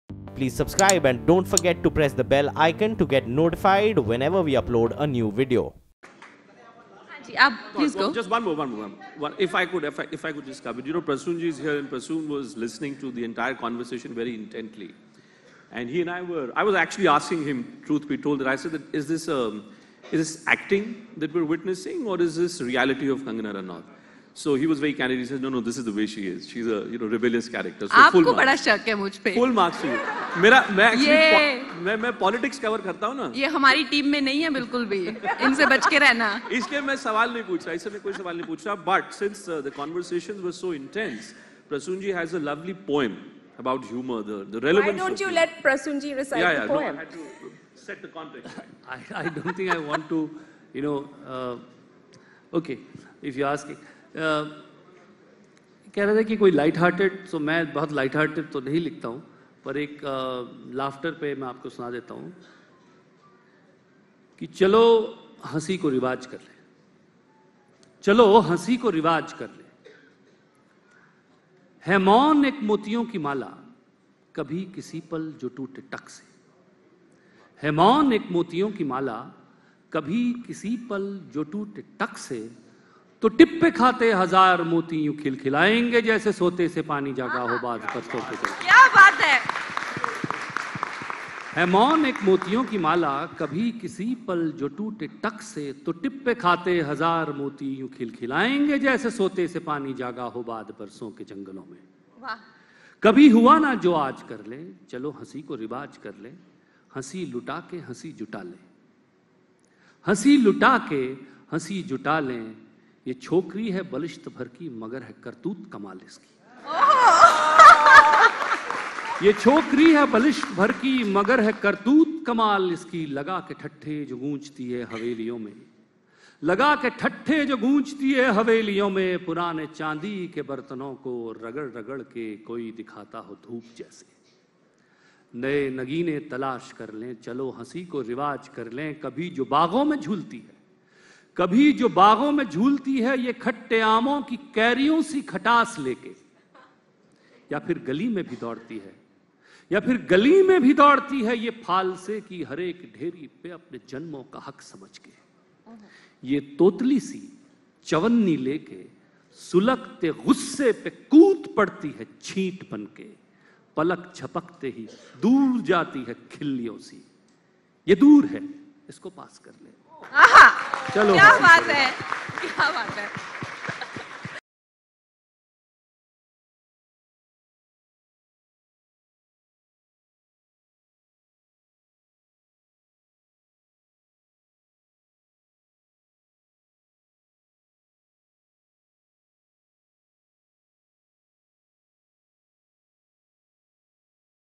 Kangana Ranaut का चारित्रिक वर्णन Prasoon Joshi की ज़ुबानी
News18RisingIndia: नेटवर्क18 द्वारा आयोजित न्यूज़18 राइजिंग इंडिया समिट में प्रसून जोशी ने अभिनेत्री कंगना रनौत के चरित्र एवं उनकी ज़िंदगी से जुड़ी घटनाओं का वर्णन करते हुए एक कविता कही. देखिये ये वीडियो.
Prasoon Joshi recites a poem on Kangana Ranaut's alleged rumors & describes her personality at News18RisingIndia Summit. Watch to know more!